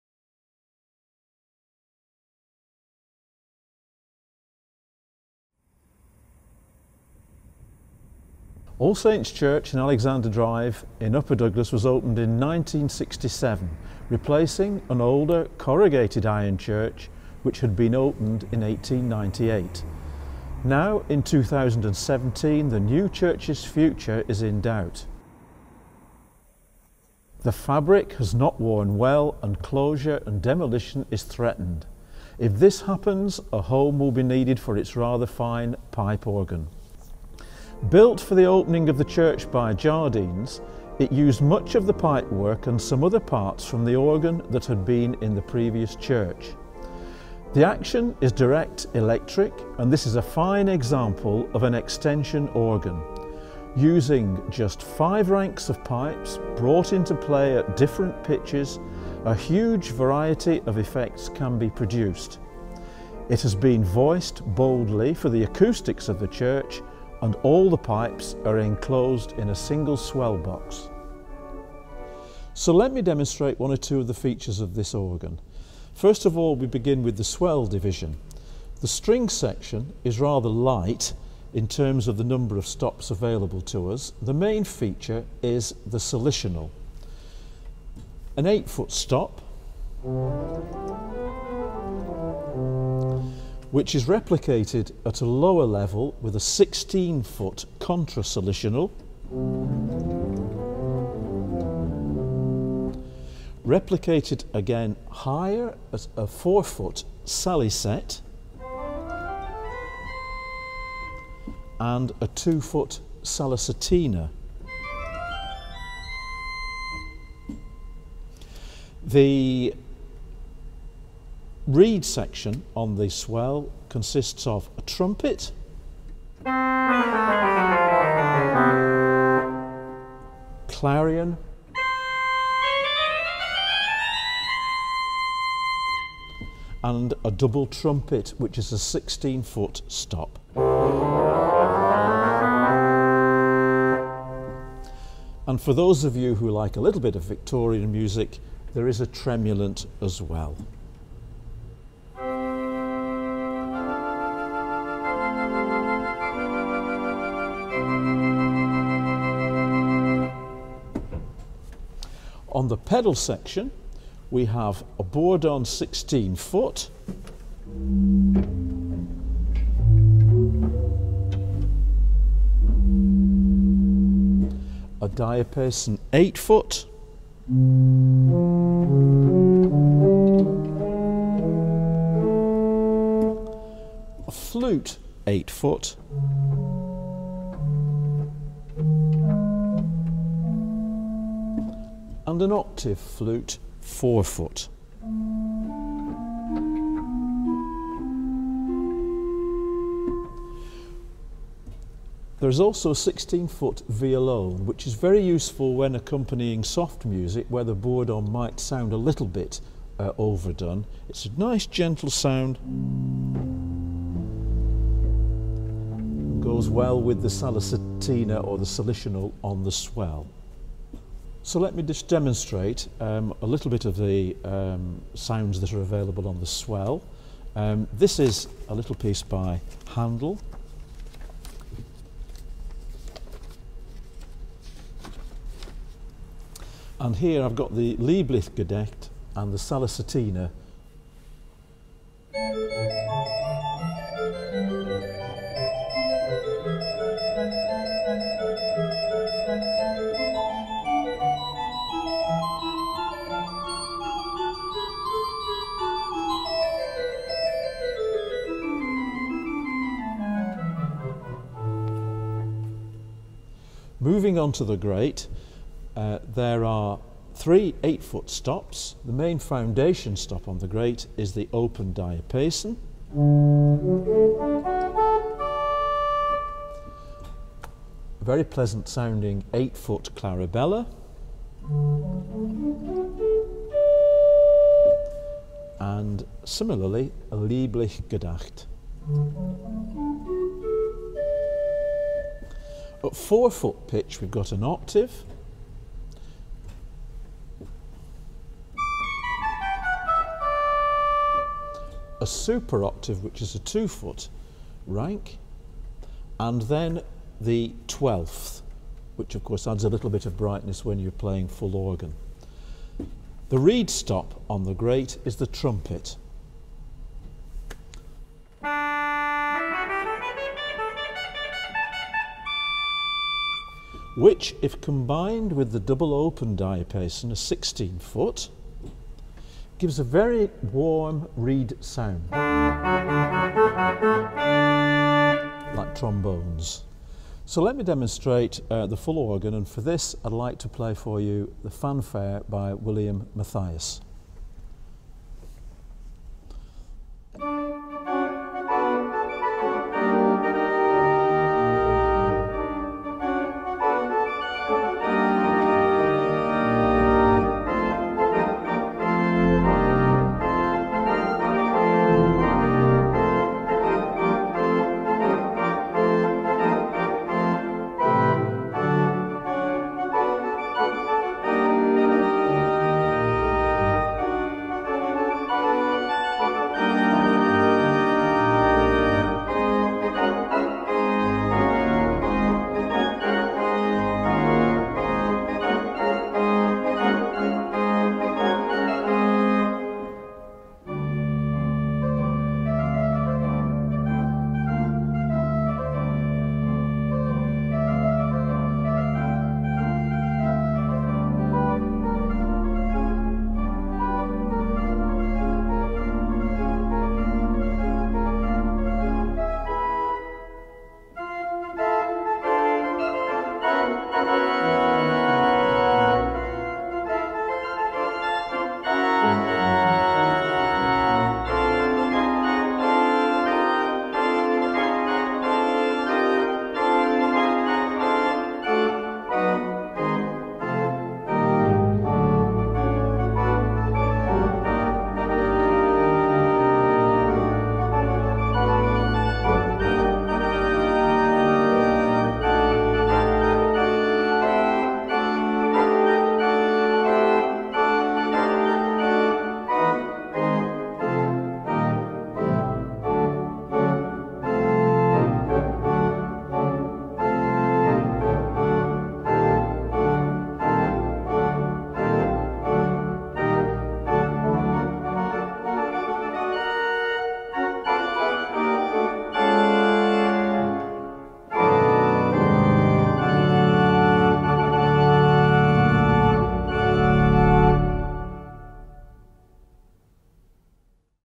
ORGAN GIVEAWAY 2: All Saints' Church, Isle of Man, UK.
All Saints' Church, Douglas, on the Isle of Man, is due to close this coming Easter.  It's 4  1/2-rank, 2-manual and pedals, unit pipe-organ will then be looking for a good home.  At the time of this edit (March, 2017) The Erin Arts' Centre is looking to raise the money needed to keep the organ on the island, but this is by no means certain.
The organ was built in the mid-1960s, incorporating some pipework from a previous instrument, and has a direct electric action.
All ranks are enclosed in a simple, brick-shaped swell-box, and the voicing is bold, but as the swell-box is very effective, the organ has a wide range from quiet to loud (possibly too loud at the console.)
The ranks are: Gedact (16' - 4') Diapason (8' - 2') Salicional (8' - 2') Trumpet (8' - 4') and a part-rank Clarabella, from tenor A, borrowing its bass from the Gedact.
  The 16' manual stops (Double Trumpet, Contra Salicional and Double Diapason) are Tenor C compass, only.
  The pedal Violone is taken from the Salicional, down to C13 on the pedalboard, then, for the last 12 notes, the 8' octave repeats, with the addition of the 16 Bourdon.
The organ is in basically good condition.  It does, however, have its original electro-mechanical switching (KA multicontact switches) which sometimes show odd missing notes.  These are easily dealt with by using spray-on switch cleaner at tuning visits.  The best solution would be a new solid-state switching system, but this is not essential.
It is hoped to keep the organ on the Isle of Man, and there is one possible home here, though this may or may not prove to be an option, depending on funds.  Any decision on the fate of the organ rests with the people at All Saints' Church, but all interested parties will be followed up and passed on to the Church authorities.
Whoever takes on the instrument will have to find the funding for its dismantling (estimated at £3,000 - £5,000, including boxing) in addition to transportation, and assembly, of course.
With many thanks to the makers of this video, Charles Guard and Graham Kirkland. (You may also wish